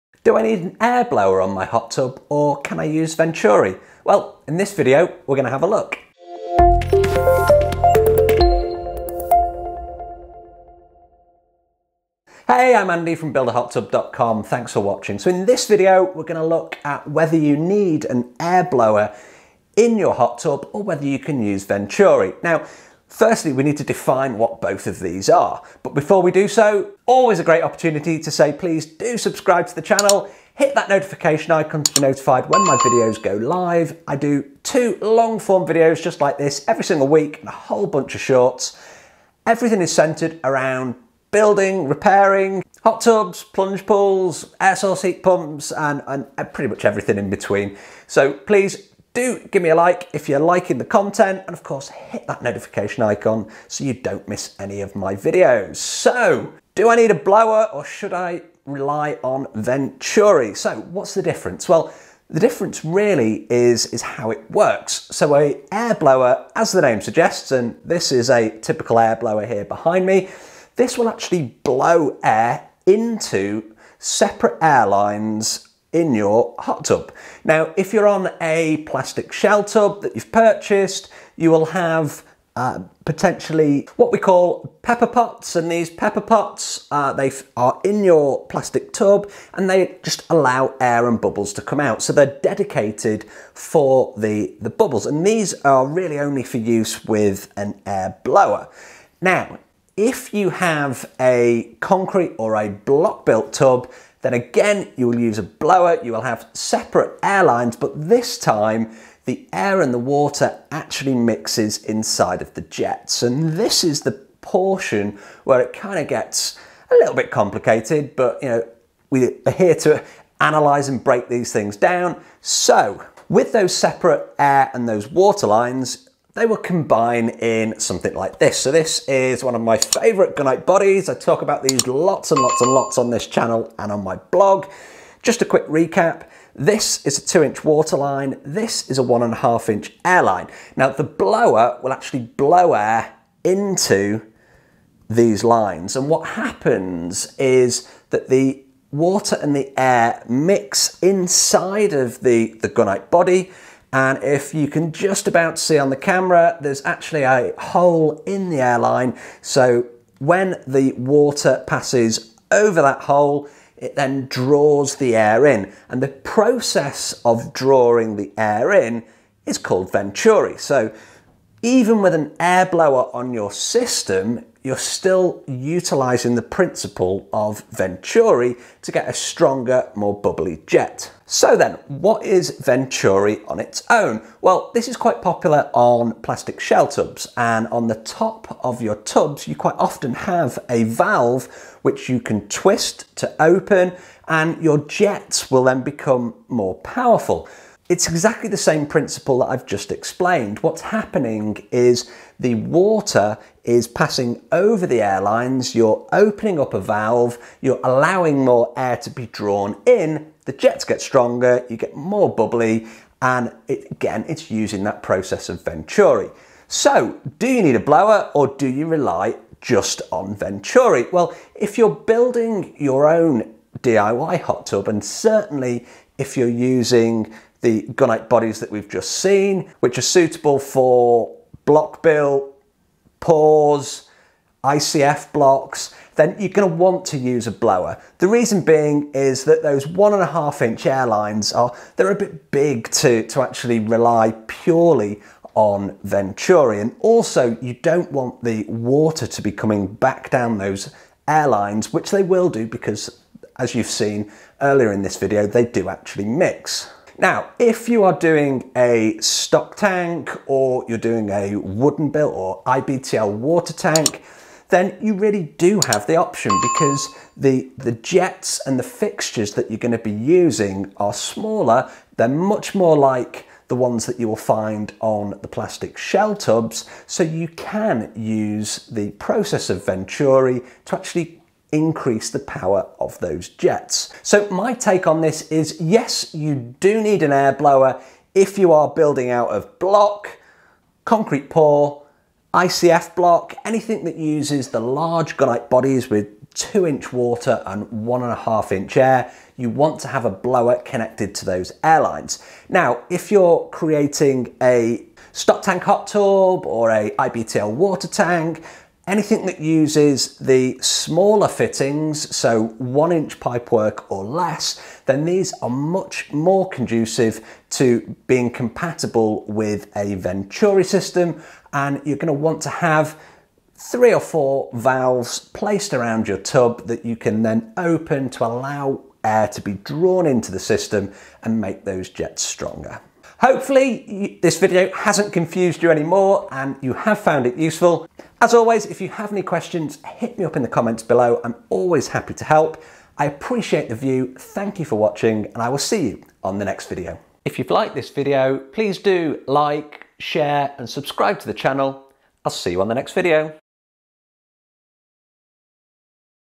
Do I need an Air Blower or Venturi on my Hot Tub?
In this video I will explore the question of whether you need an air blower or venturi on your hot tub. If you’re a hot tub owner, you may have heard these terms before and wondered what they are, or if they’re even necessary. Likewise, if you are in the design stages of a new hot tub you may be wondering what to do.The truth is, both of these components can enhance your hot tub experience, but it depends on your personal preferences and the specific features of your hot tub. In this post, I’ll break down the differences between air blowers and venturis, and provide you with the information you need to decide which one, if any, is right for your hot tub.Whether you’re a seasoned hot tub owner or new to the world of hot tubs, this post will give you a better understanding of these components and help you make an informed decision. So, let’s dive in!What is Venturi for hot tubs jets?A Venturi is a component that can be added to hot tub jets to enhance the water flow and create a more vigorous massage experience. It works by using the pressure of the water flowing through the jet to create suction, which draws in air and mixes it with the water.This process results in a powerful, swirling water flow that can provide a deep tissue massage and help relieve sore muscles.The Venturi is typically installed on the front face of the jet, and can be adjusted to control the amount of air that is mixed with the water. This allows hot tub owners to customize their massage experience, by either increasing or decreasing the intensity of the water flow. This is done with an air valve that is usually found on the top of the tub While not all hot tubs come equipped with a Venturi, they can be added as an aftermarket accessory. However, it’s important to note that not all hot tub jets are compatible with Venturis, so it’s important to check what kit you actually have inside. You can always send me a picture if you are not sure!Additionally, some hot tubs may have a built-in Venturi system that is activated by a separate control knob or button on the hot tub’s control panel.Overall, a Venturi can be a valuable addition to hot tub jets for those who prefer a more intense massage experience. What is a hot tub air blower and how does it work? A hot tub air blower is a component that can be added to a hot tub to create a bubbly and effervescent water flow. It works by using a motor to suck in air and then force it through a pipe, which is connected to small air jets in the hot tub. These air jets then release the air into the water, creating thousands of tiny bubbles that can create a relaxing and rejuvenating experience. Hot tub air blowers are typically located outside of the hot tub and can be controlled with a separate switch or button on the hot tub’s control panel. This allows hot tub owners to customize the amount of air that is released into the water and adjust the intensity of the bubbly water flow. One of the main benefits of a hot tub air blower is that it can provide a gentle massage experience that is less intense than a jet massage. The bubbles created by the air blower can help to soothe sore muscles and promote relaxation. Additionally, the air bubbles can help to distribute heat more evenly throughout the hot tub, which can result in a more comfortable and enjoyable soaking experience. While hot tub air blowers can be a valuable addition to a hot tub, they are not for everyone. Some people may prefer a more intense massage experience provided by hot tub jets, while others may find the noise of the air blower motor to be disruptive to their relaxation. Additionally, some hot tubs may have a built-in air blower system, while others may require an aftermarket accessory to be installed. I’m building my Hot Tub out of Block or Concrete – should I use a blower or venturi? The Gunite Bodies which are used in this type of hot tub build, are designed to work with a blower. There are two pipe fittings per jet, a 2” water line and a 1.5” air line. The water and the air mix inside of the Gunite body through the process of venturi.However, unlike the process described above, the Gunite body is expecting the air to be blown into the pipes rather than drawn in. Given the size of the air lines on this style of hot tub build, they are not suitable for a pure venturi process, they need a blower to be attached. Can I upgrade a venturi system to a blower? In the most basic terms, the answer is yes, you can. However, in order to do so, you will need to cap off the air valves that are used for the venturi process. The main difference between a venturi system and an air blower is that the pipework for an air blower is a closed system so pressure builds. For venturi, it is open as you are opening valves to let the air in. If you open a valve whilst the blower is running you will lose pressure and also the noise will increase – not what you want on a hot tub.